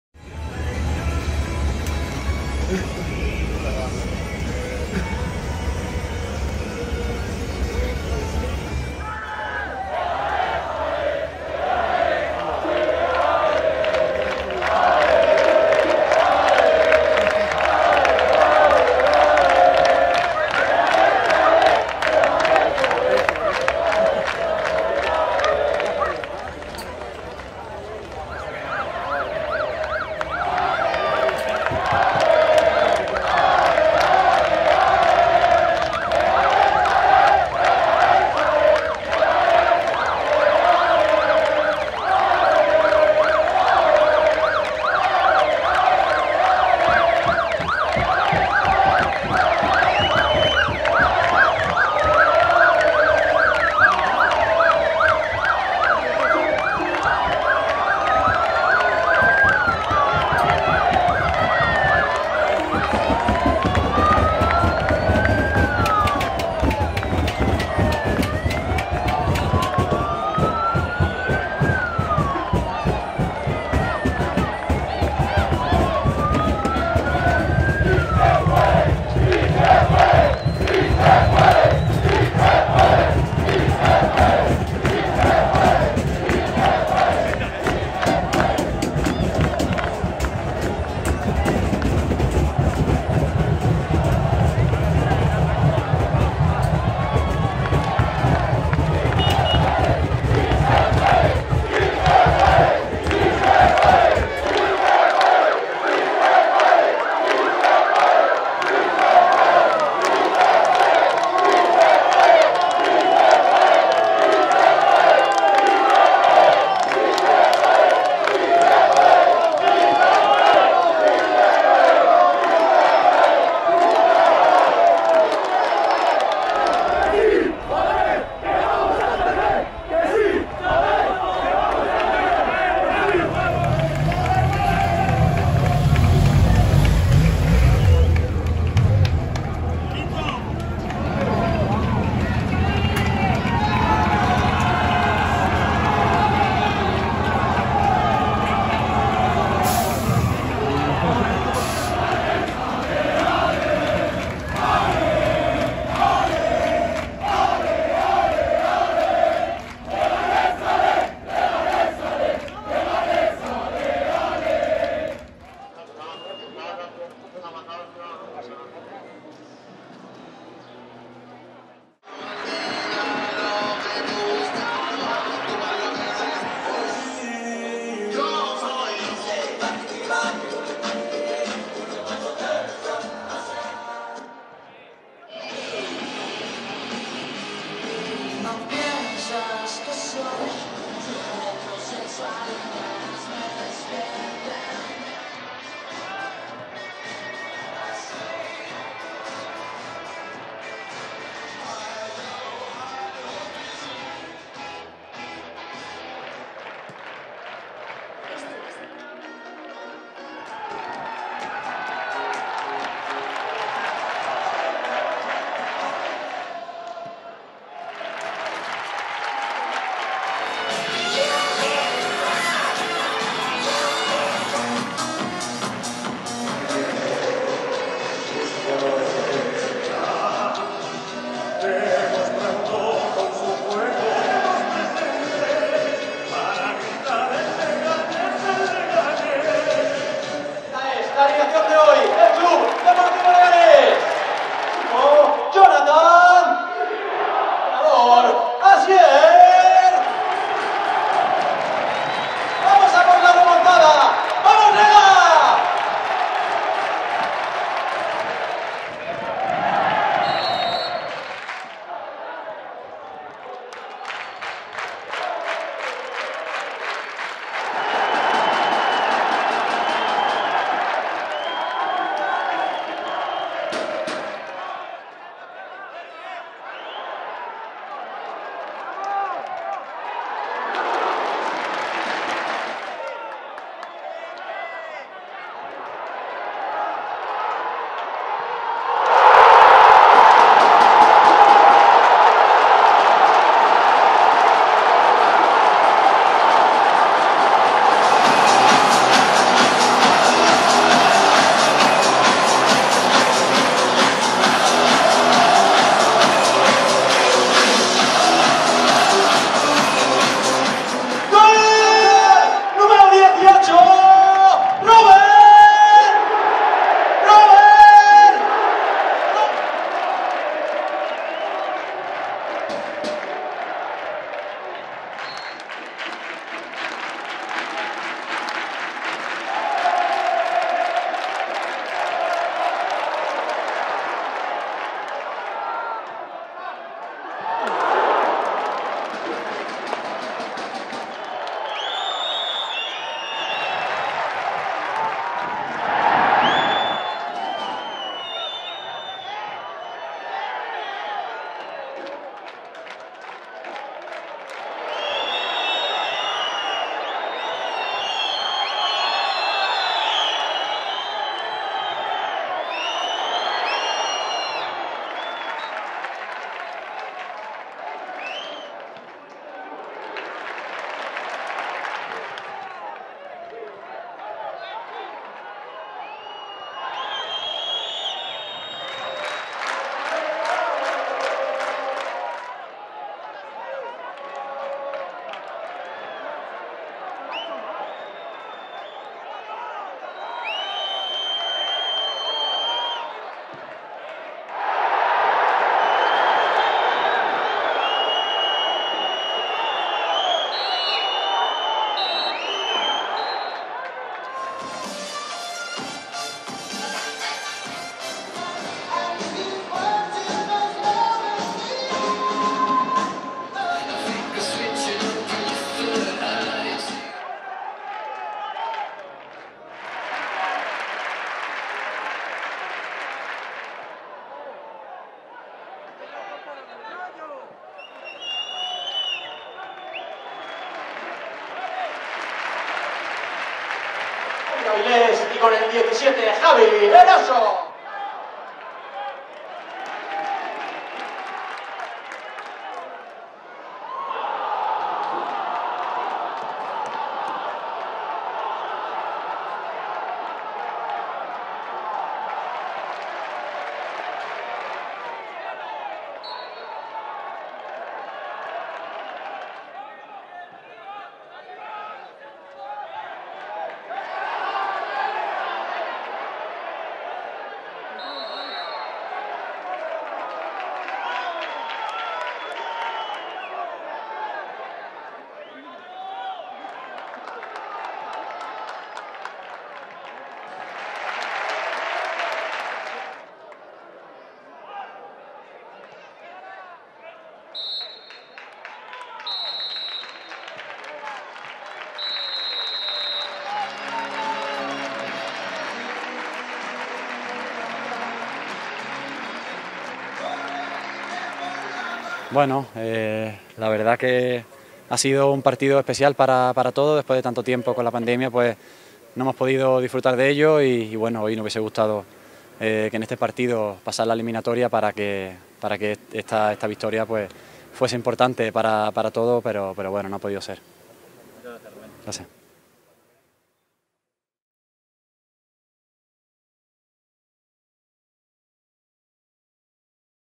1.500 AFICIONADOS APOYARON AL C.D. LEGANÉS ANTE EL RAYO VALLECANO
El C.D. Leganés contó con el apoyo de su afición en el partido ante el Rayo Vallecano. 1.500 pepineros alentaron al equipo en Butarque y otros muchos lo hicieron antes del partido recibiendo al autobús a su llegada al estadio.